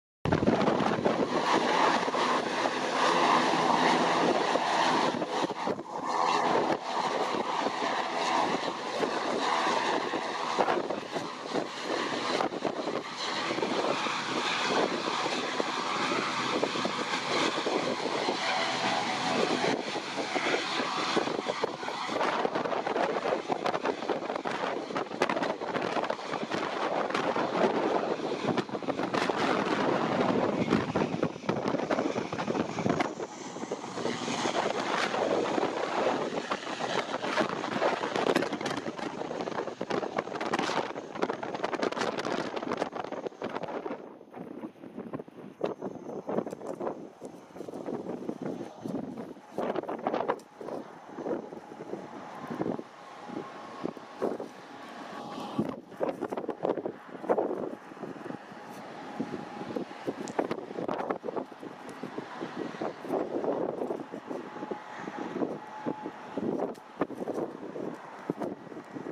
Wellington Airport | Air New Zealand | ATR 72-600 (ZK-MVM) take off RWY34
credits---James Raven